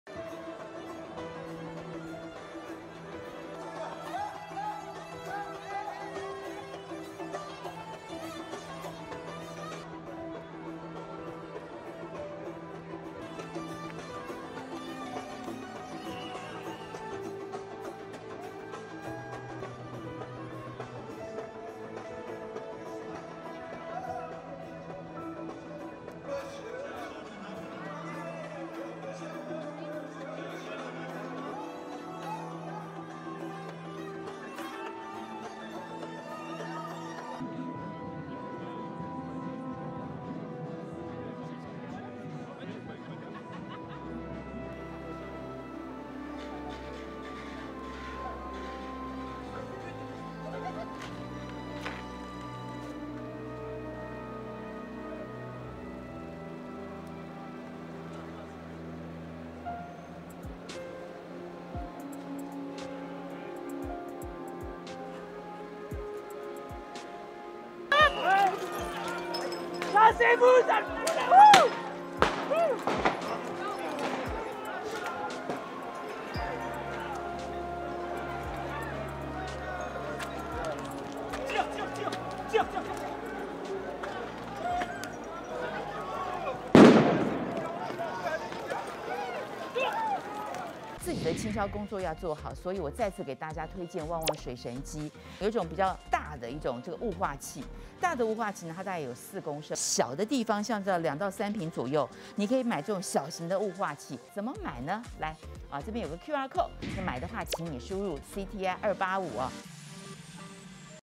戰爭讓勞工日子更苦 巴黎人上街頭抗議 銀行.速食店遭砸爛 5千員警鎮暴｜全球線上 @中天新聞 ​
責任小編 wei編cti888/快馬加編